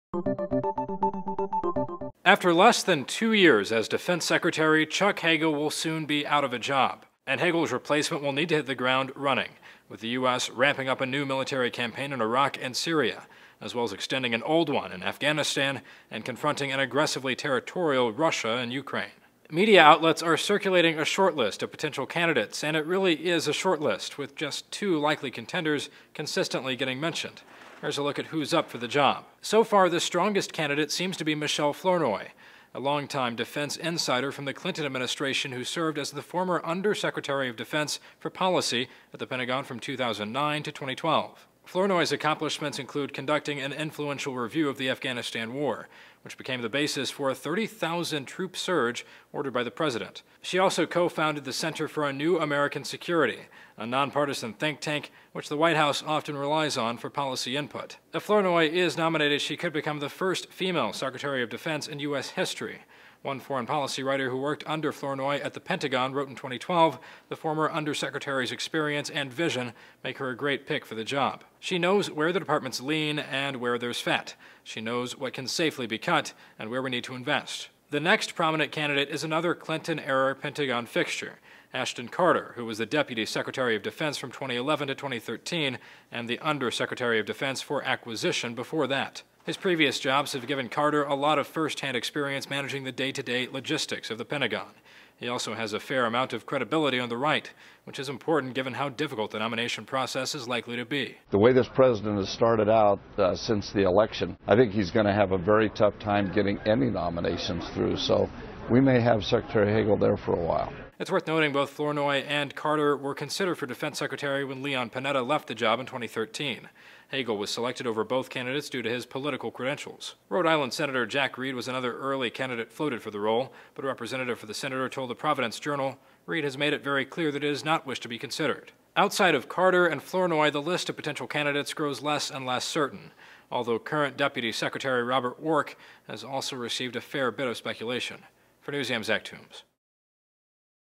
As Hagel Steps Down, Two Likely Replacements Emerge
Defense Secretary Chuck Hagel announced his resignation Monday, and two potential candidates to replace him have already emerged.